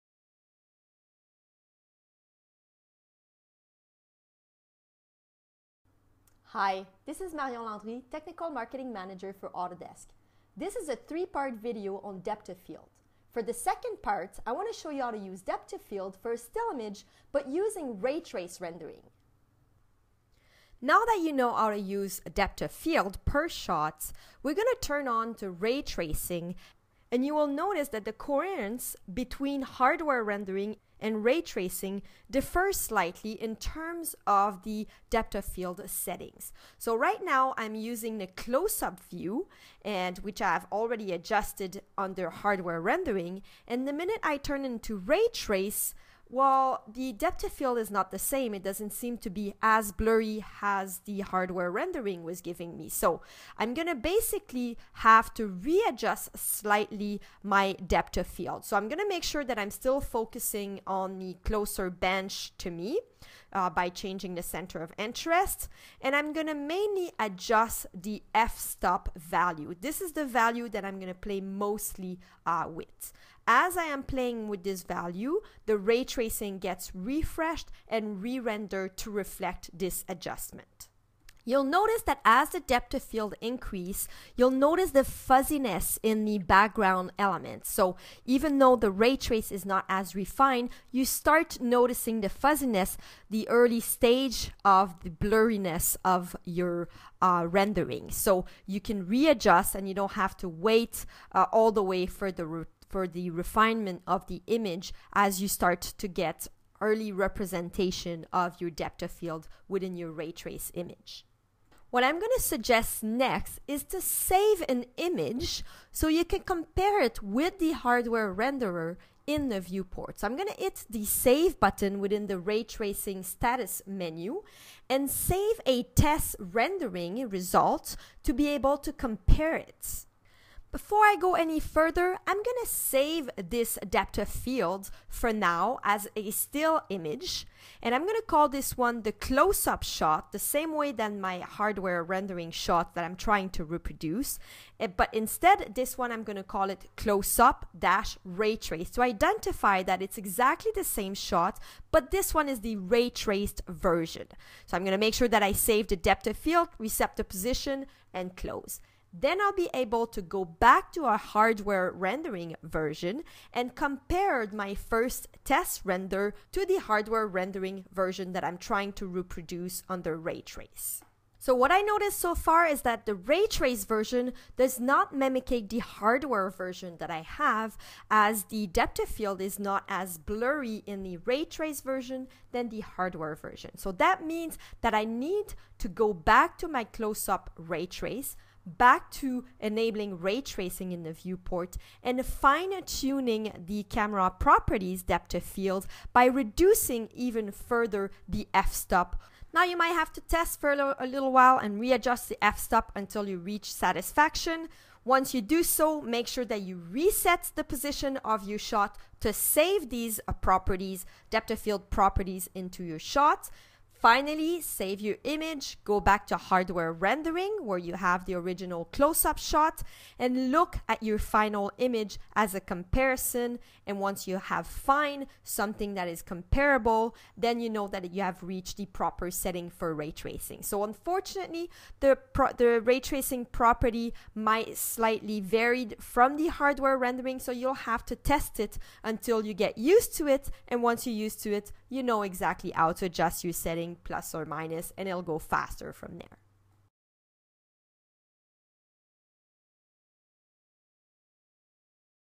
Showcase Tips & Tricks: Showcase 2014 Depth of Field Part 2
Still Shot Using Ray Traced Rendering: This is a 3 parts video on Showcase depth of field (DOF). For this video, learn how to use DOF in combination with Ray Tracing. DOF in hardware rendering is a post processing option and is not as accurate then in Ray Tracing mode. That means that the coherence between Hardware Rendering and Ray Tracing might differ. Make sure to watch the second and third part of this series to learn more about DOF.